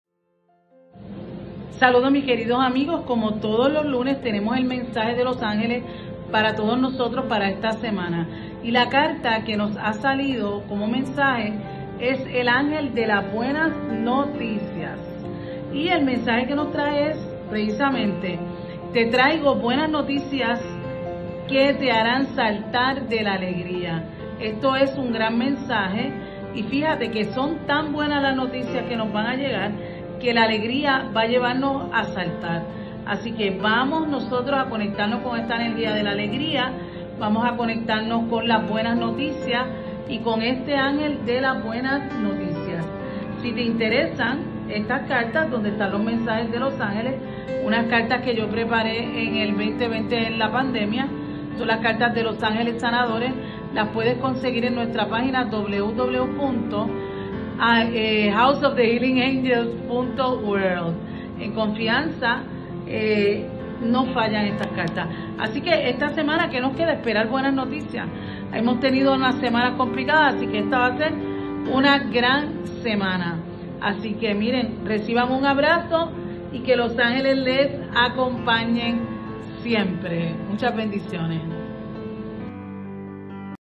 💌 Mensaje de los Ángeles para la Semana del 14 al 21 de Noviembre ✨ / Joyce Pagán
Mensaje de los Ángeles para la Semana del 14 al 21 de Noviembre!

✨El Ángel del las Buenas Noticias ✨ Te traigo Buenas Noticias que te harán Saltar de la Alegría 🙌🏼

🙌🏻 Que los Ángeles los acompañen Siempre!!!

✨Conectáte conmigo✨

🌐 Visita mi página web para ver nuestros productos y servicios